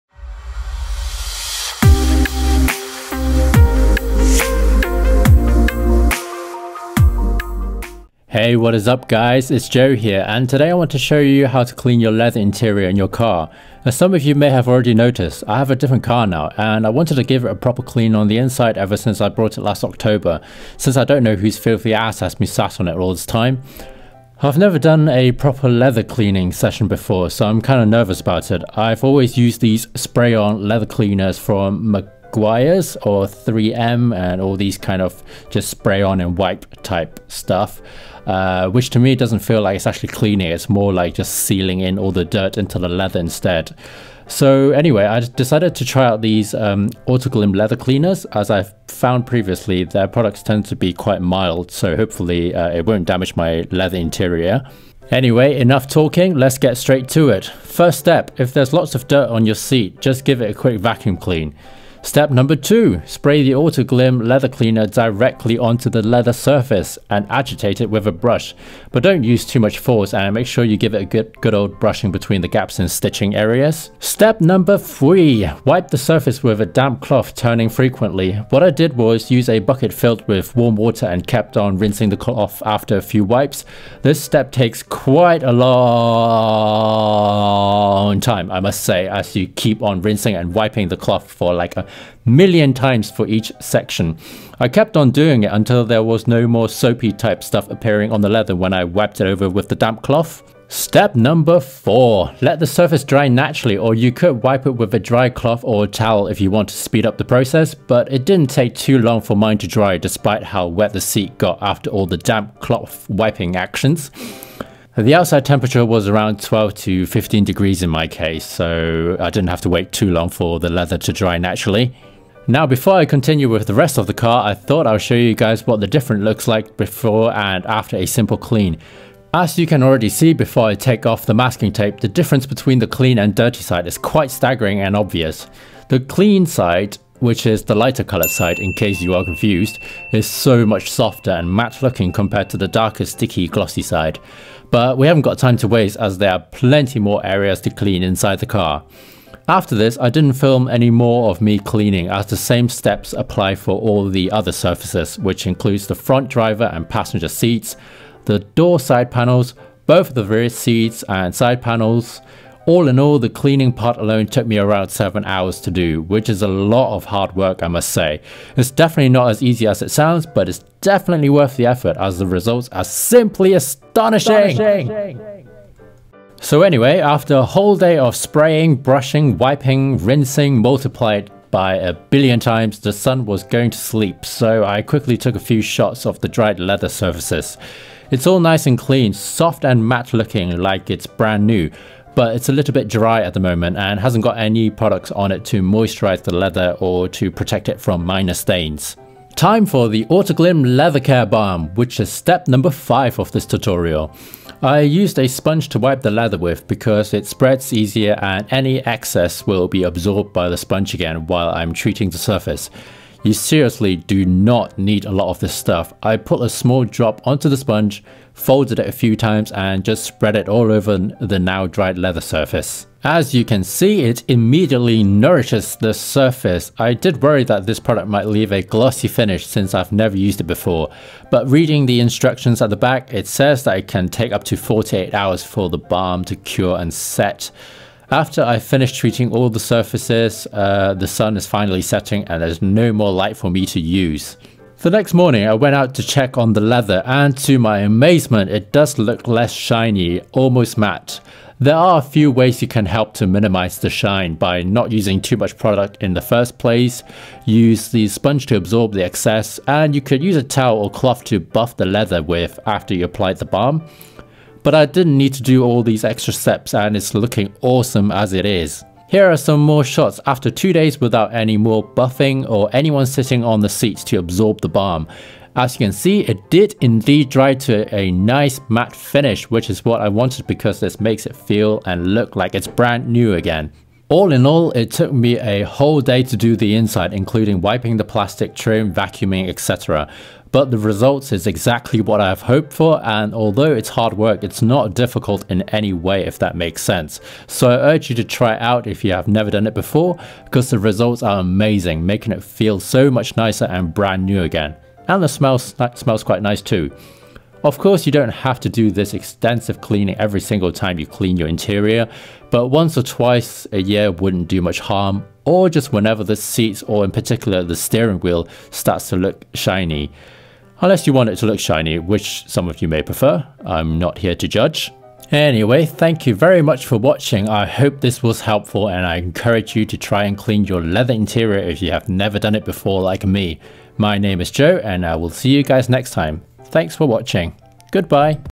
How to clean your leather interior - Autoglym Leather Cleaner / Care Balm
Does your interior leather look glossier than some photo paper?
Time to clean off all the dirt, grime and oil with this step by step easy to follow tutorial.

BMW M3 E92 used for demonstration.

Autoglym Leather Clean & Protect Kit

Meguiar's Quik Interior Detailer Cleaner (Matt Finish) for interior trim / dashboard

GEAR I USE TO MAKE VIDEOS